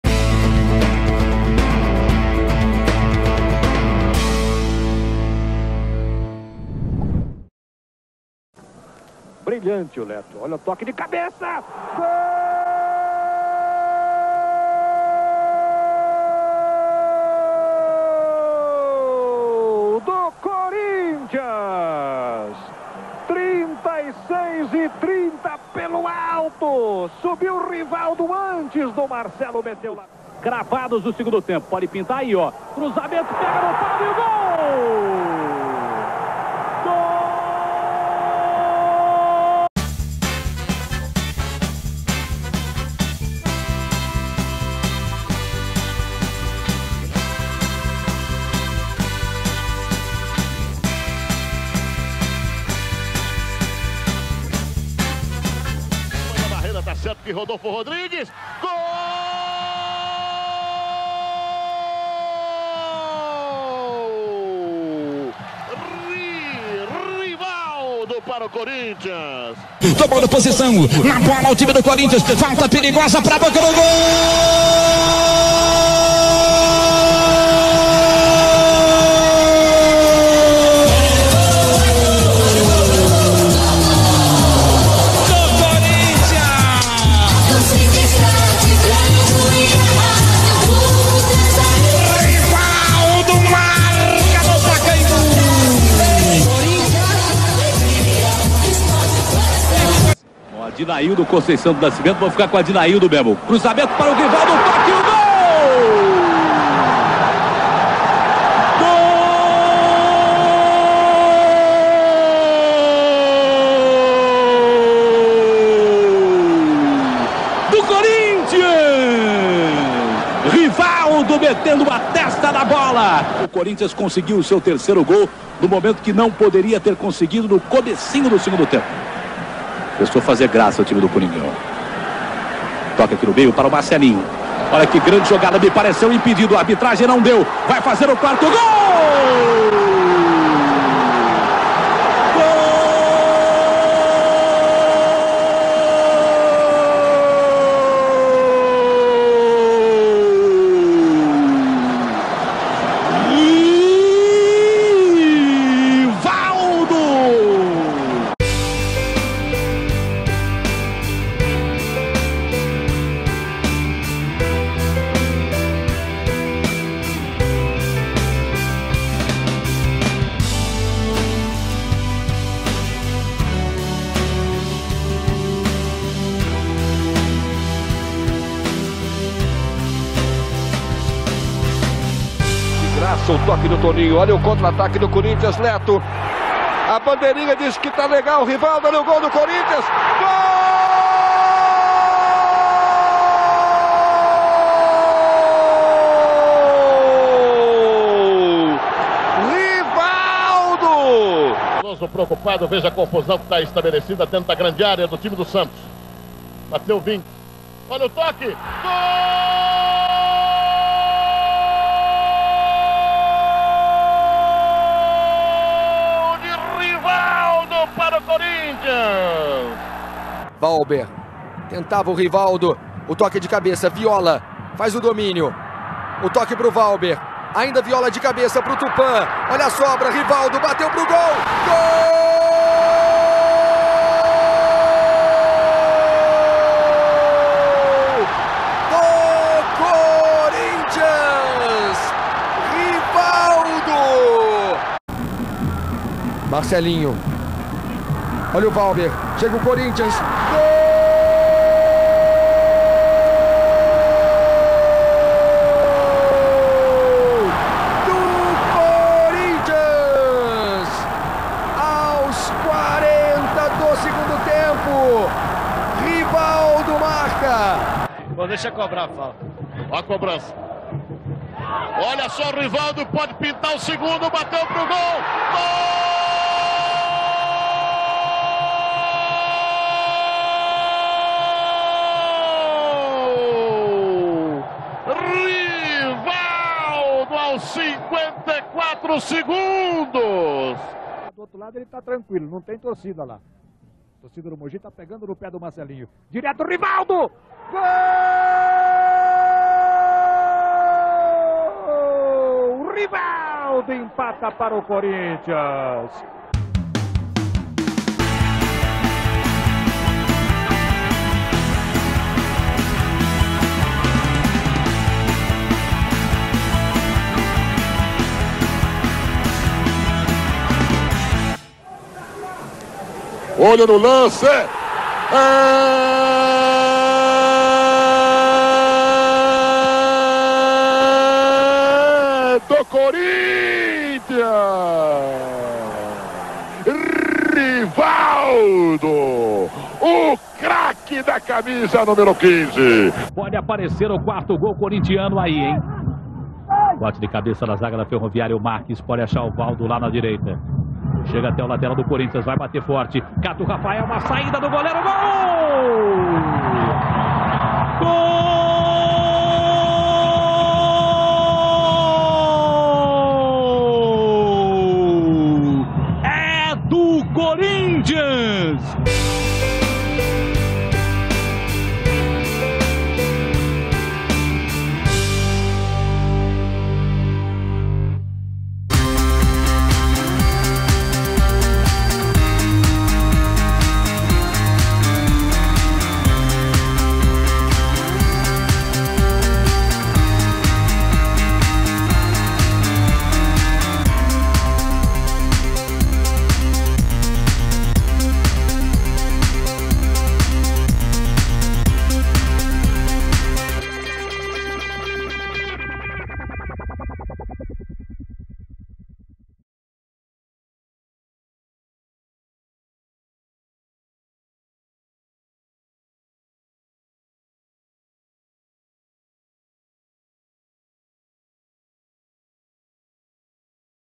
Meia Rivaldo! Gols pelo Corinthians!
O meia Rivaldo, melhor jogador do mundo na temporada de 1999 teve sua passagem pelo Corinthians nas temporadas de 1993 e 1994, após se destacar no Mogi Mirim ele chega ao Corinthians, em 62 jogos o meia marcou 22 gols, no vídeo temos imagens de 19.